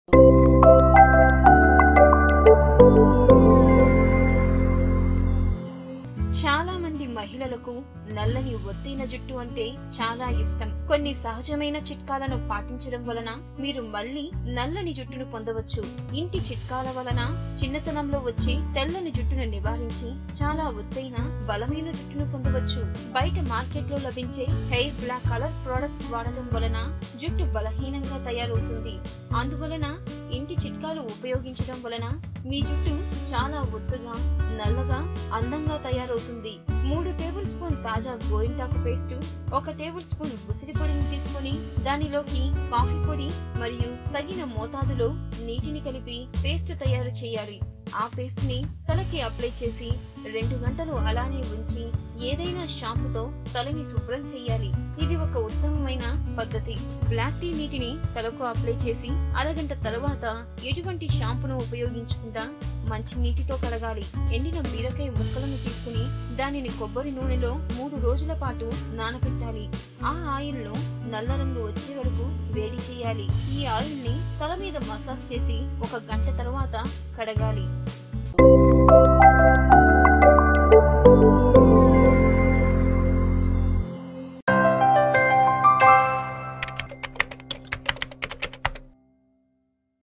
10 easy Homemade Tips for Strong and Smooth Hair Growth in TELUGU| NH9 News
10 easy Homemade Tips for Strong and Smooth Hair Growth in TELUGU